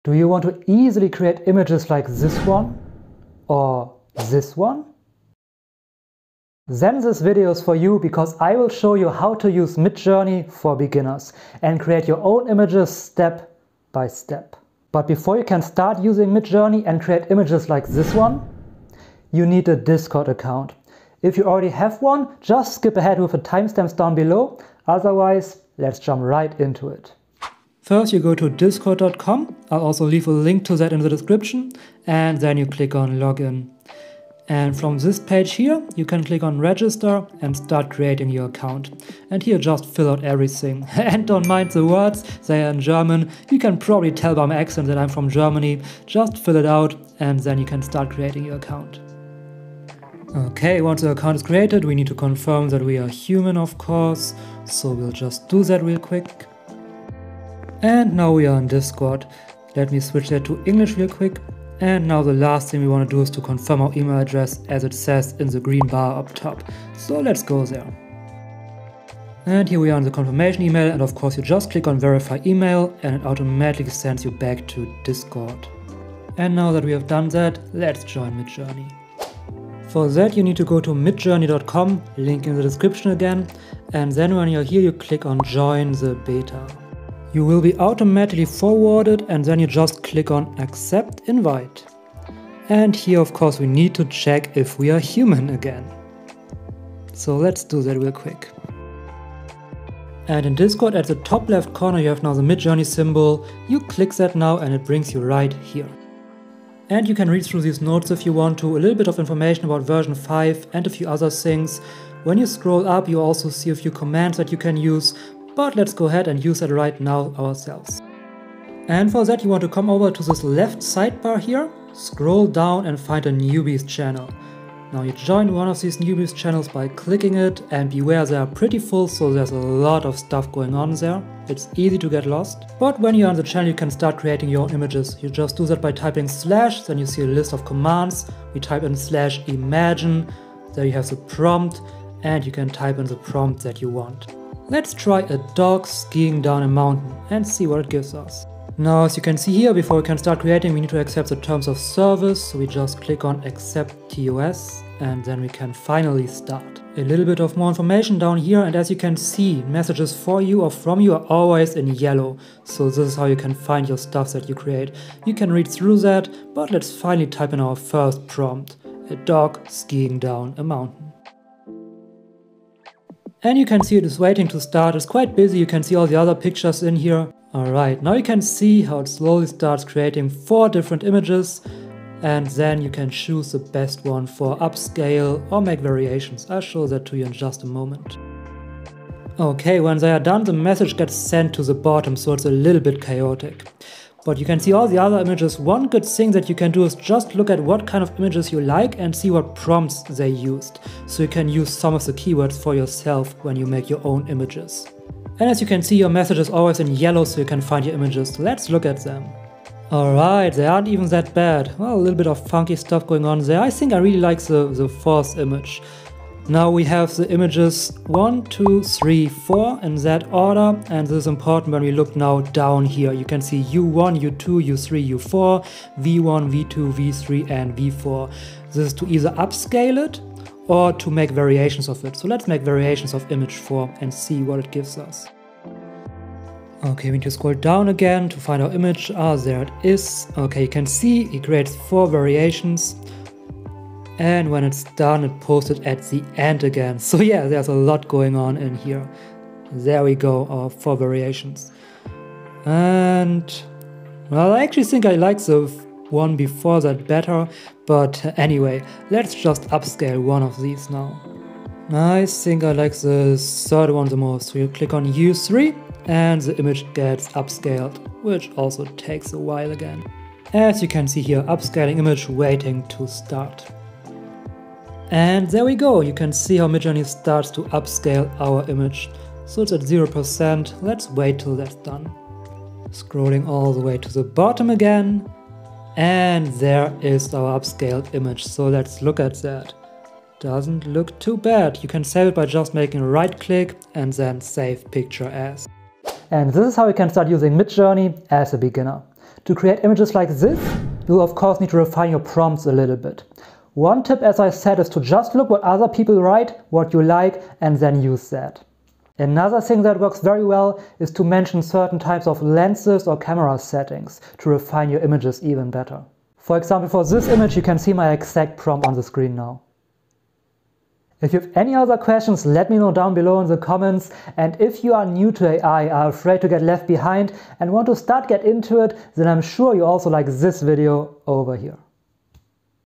How to use Midjourney (for Beginners)
learn how to use Midjourney for beginners. I'll cover creating a Discord account, signing up for Midjourney, the interface, and how to make your first prompt. If you are a beginner, have never used it before, and maybe even fear getting left behind, this is perfect for you.

0:00 Intro
0:27 Discord Account Creation
1:20 Joining Midjourney
2:00 First Prompt
3:37 Upscaling and making Variations
4:56 Tips + Outro